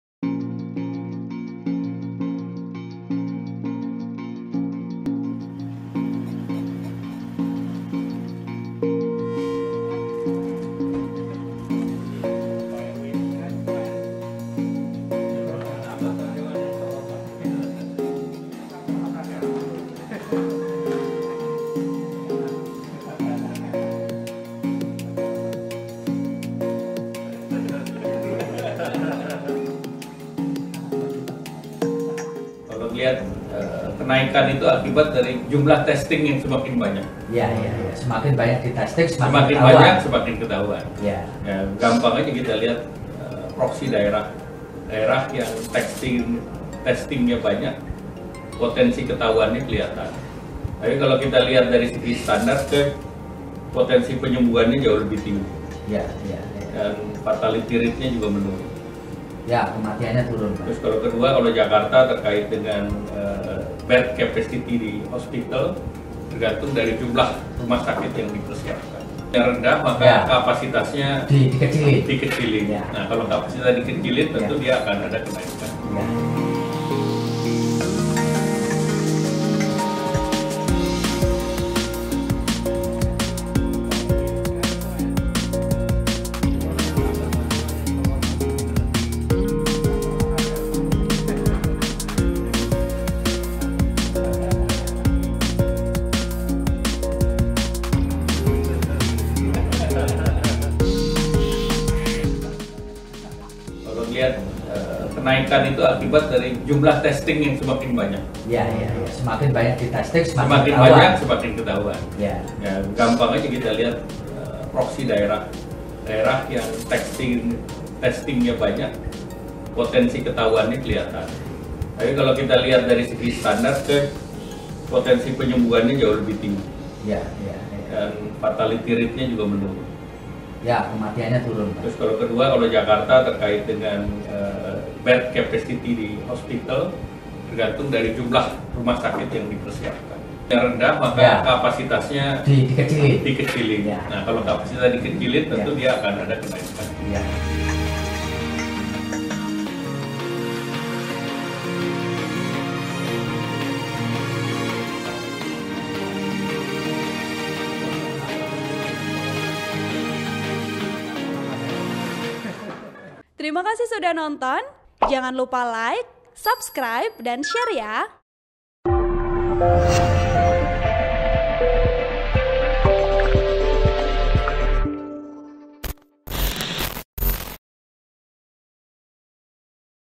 Mengenai Melonjaknya Kasus Penularan Covid 19, Begini Kata Menko Perekonomian Airlangga Hartarto
TRIBUN-VIDEO.COM - Melonjaknya kasus penularan Covid-19 disebabkan karena semakin banyaknya jumlah orang yang dites.

Hal tersebut dinyatakan Menteri Koordinator (Menko) Perekonomian Airlangga Hartarto di kantor Tribunnews, Jalan Palmerah Selatan, Jakarta Pusat, Selasa (1/9/2020).

Airlangga Hartarto mejelaskan, semakin banyak oranng yang melakukan tes, semakin banyak yang ketawan mengidap Covid-19.

"Semakin banyaknya penambahan kasus itu karena semakin banyak yang dites. Kalau makin banyak yang dites kan makin banyak yang ketawan," kata Airlangga Hartarto.

Airlangga Hartarto menambahkan, Indonesia memiliki tingkat penyembuhan yang jauh lebih tinggi dibanding tingkat kematian maupun penambahan kasusnya.

Oleh sebab itu, penanganannya saat ini diperhatikan terus fasilitas kesehatan untuk penyembuhan Covid-19.

Contoh di DKI Jakarta kapasitas tempat tidur di rumah sakit untuk pasien dikurangi atau dialihkan untuk pasien non-Covid-19, karena pasien yang dirawat tidak terlalu banyak.

Sebagaiaman diketahui, penambahan kasus baru penularan Covid-19 pada satu hari lalu mencapai lebih dari 2000 kasus.

Sehingga total jumlah kasus Covid-19 di Indonesia sebanyak 174.796 dengan tingkat kesembuhan mencapai 128.057 dan tingkat kematian mencapai 7.505.